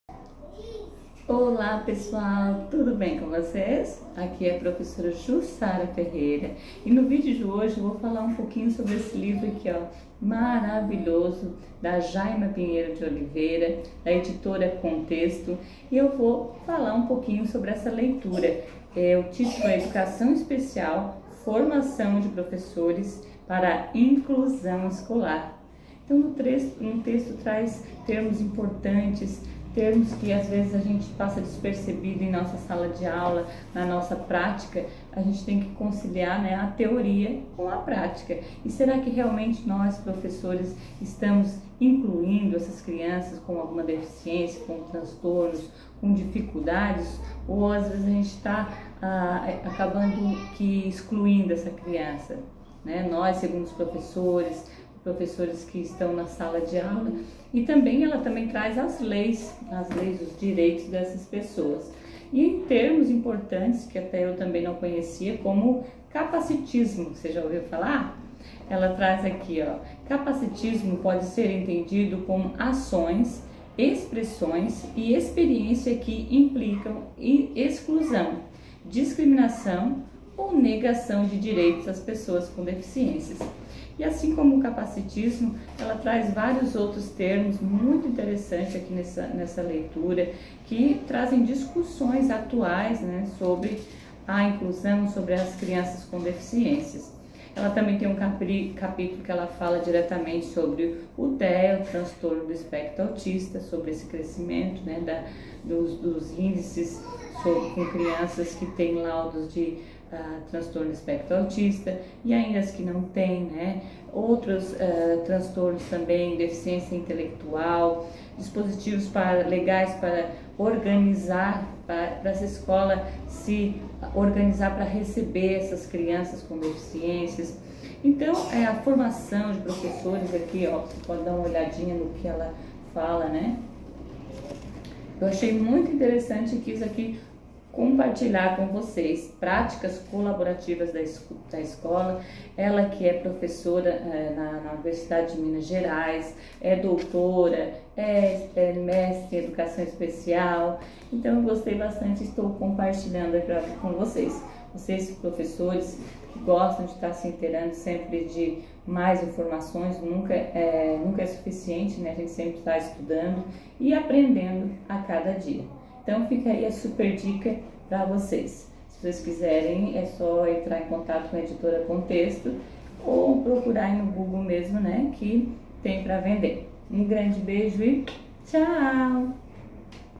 Livro: EDUCAÇÃO ESPECIAL: Formação de professores para a inclusão escolar. Ed. Contexto
Livro com um texto muito importante para professores e profissionais que trabalham com pessoas com deficiência, que queiram se atualizar sobre o tema. Educação especial.